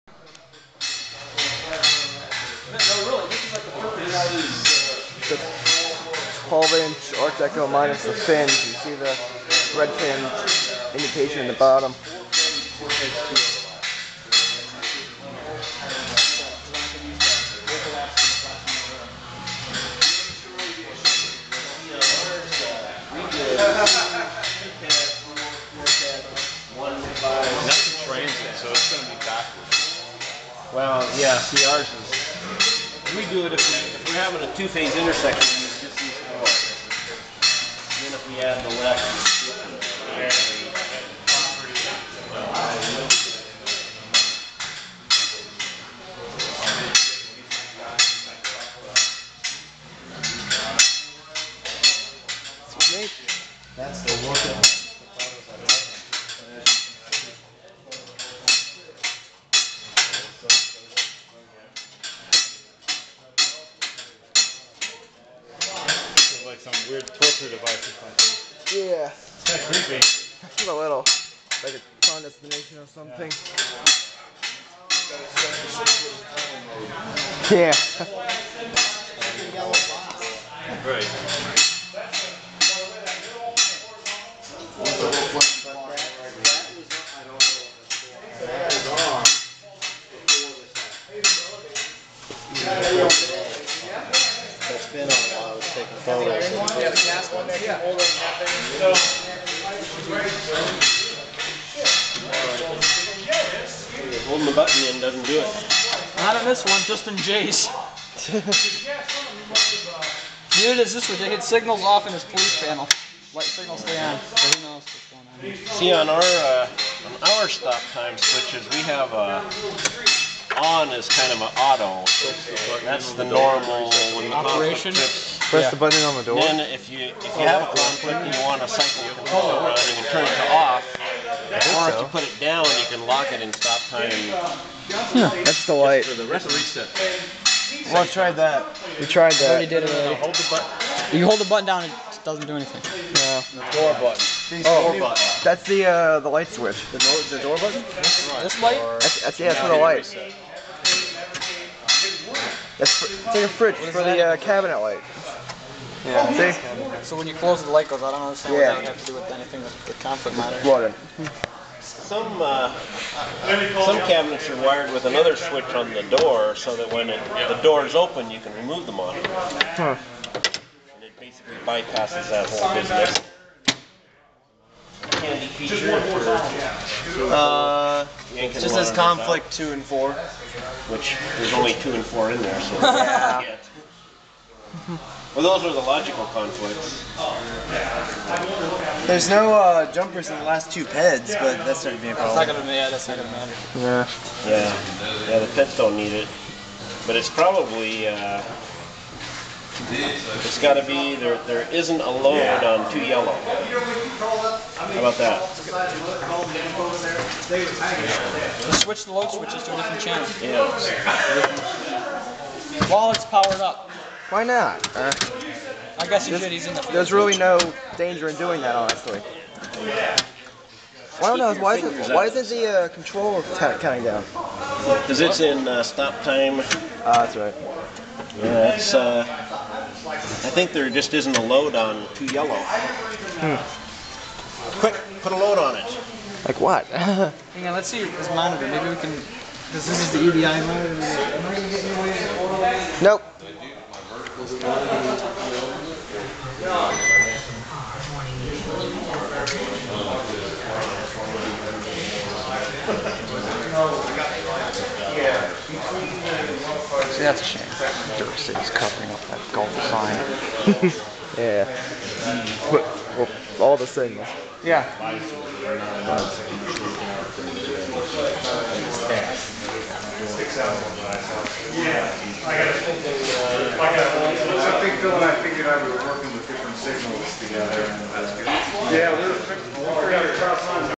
Lilteach's pole barn
Walking around Dave/Lilteach's pole barn. Can you say TRAFFIC LIGHTS?